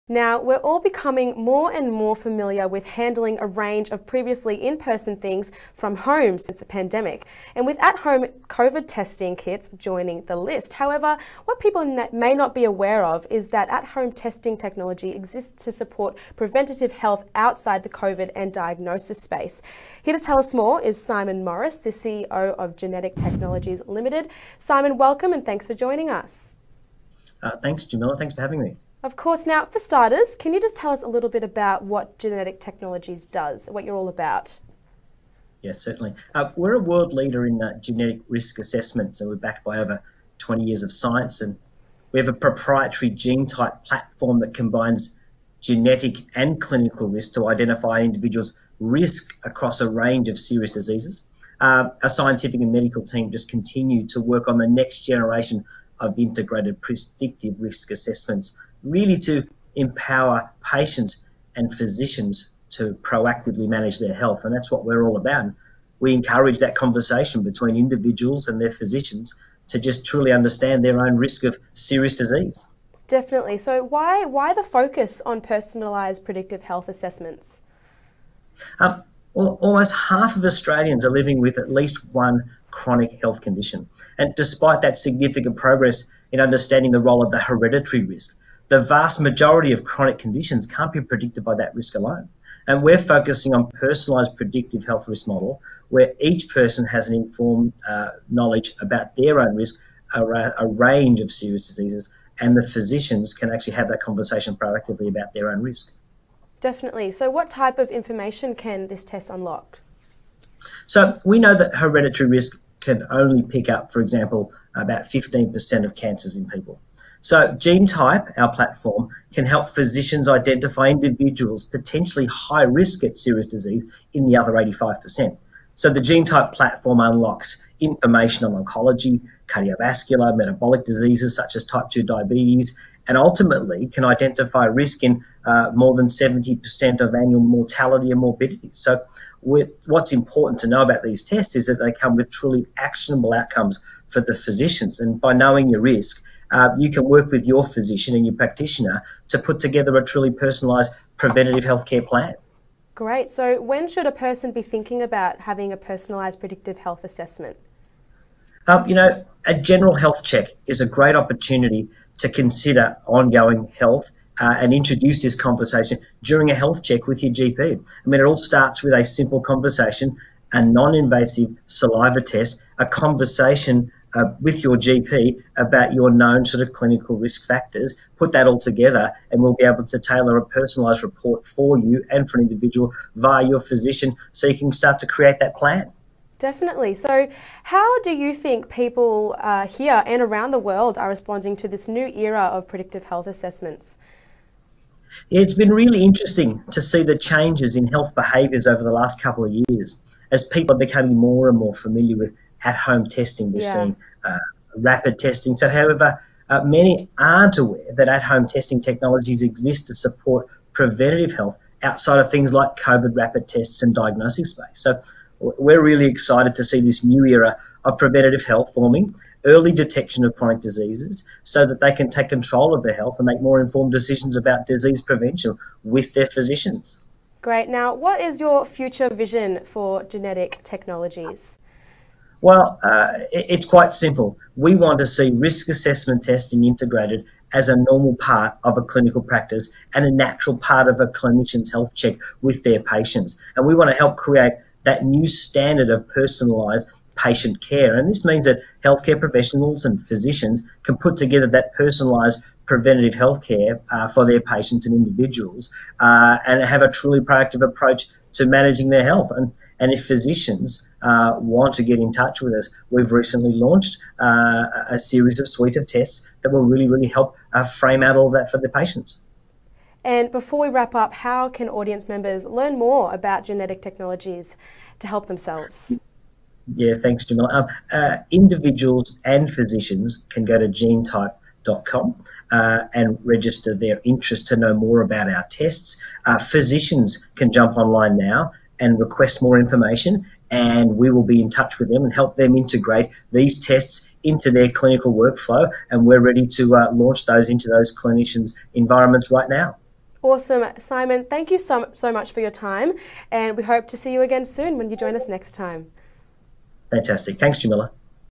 CEO Simon Morriss - Ticker News Interview (29 March 2022)
A world leader in genetic risk assessment, Genetic Technologies’ proprietary technology combines clinical and genetic risk factors to identify an individual’s risk across a range of serious diseases. Such preventative tests are now available to consumers through their medical practitioners. CEO Simon Morriss shares more with Ticker News, an international streaming news network.